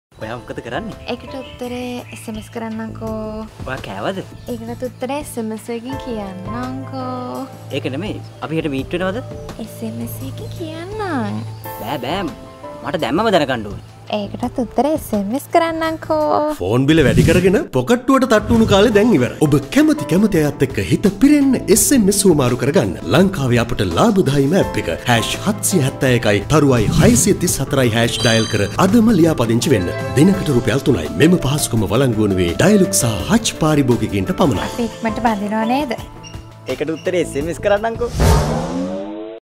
Chat Board
ඔබේ දුරකථන බිල වැඩිද? SMS වලට යන ගාස්තුව වැඩිද? Pocket එකට තට්ටු නොවි දවසෙම SMS Chat කරමුද ? මෙන්න එයට විසඳුම අපෙන්. රු.3 ක් ගෙවා හිත පිරෙන්න දවසම SMS කරන්න.ලංකාවේ අපට ලාබම APP එක .
ලියාපදිංච් වීමට Dial #771*634 # නැත්නම්
Reg(space)CBO ලෙස type කර 77200 ට SMS කරන්න. ඕනැම වර්ගයක දුරකථනයක් මේ සදහා භාව්තා කල හැක.
(මේ සඳහා ඔබ හා sms යවන්නාද මෙම සේවාවට ලියාපදිංච් වී සිටිය යුතුයි.)
මෙම සේවාව ලබා ගත හැක්කේ Dialog සහ Hutch පාරිභෝගිකයින්ට පමණි.
මේ සඳහා දිනකට රු.3+ tax අය වේ.